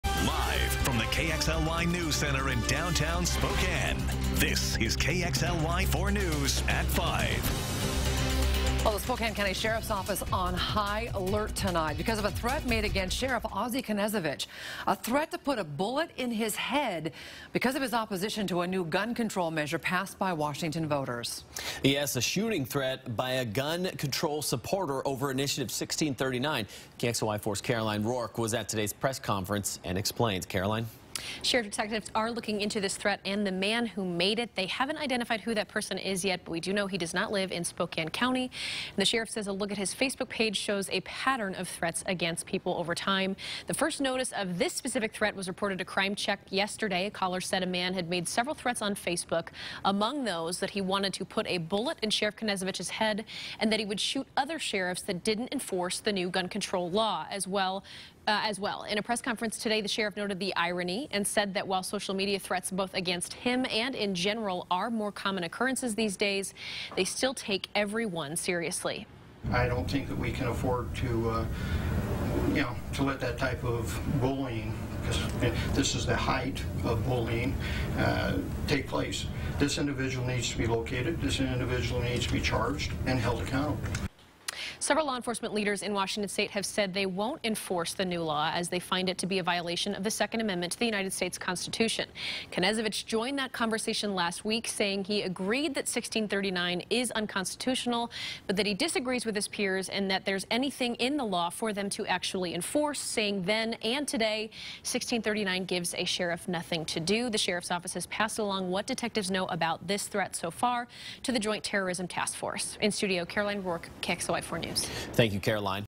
Spokane County Sheriff Ozzie Knezovich responds to death threats
The Spokane County Sheriff's Office is investigating online threats against Sheriff Ozzie Knezovich. KXLY 4's Caroline Rourke reports.